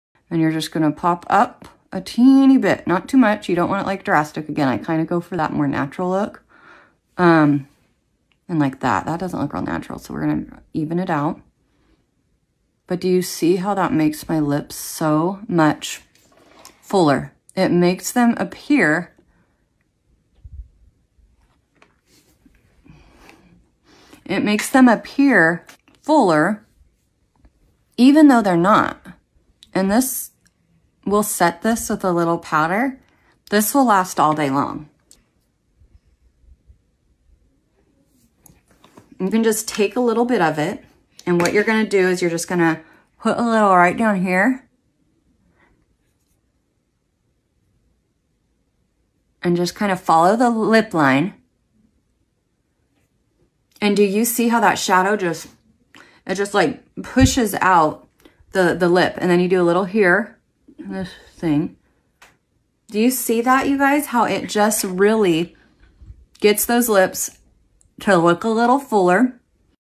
Full Face Makeup Tutorial part 10 of 13ish
Full Face Makeup Tutorial part 10 of 13ish. I know it may take a while to see this complete makeup reveal but I have to tell you one thing. It’s entirely worth it. Don’t miss the next ones or you will never enjoy the final look of perfection. I hope you feel influenced.

Follow @therealshannypants @shannypantsshowpodcast

ShannyPantsShow.com